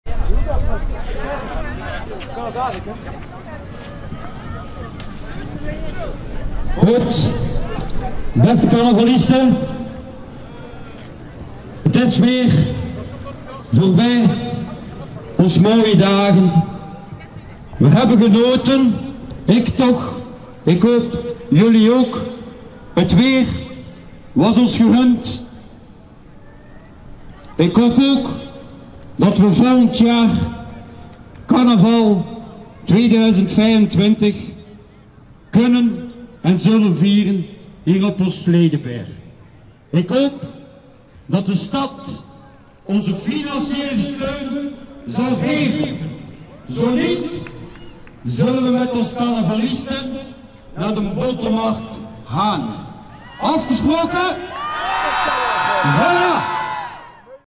hopelijk krijg karnaval Ledeberg in 2025 terug financieel steun van stad Gent
hopelijk krijg karnaval Ledeberg in 2025 terug  financieel steun van stad Gent om terug carnaval te kunnen vieren
zo niet gaan we tezamen met de carnavalisten van Ledeberg naar botermarkt gaan ( aan stadhuis van Gent om financieel steun te krijgen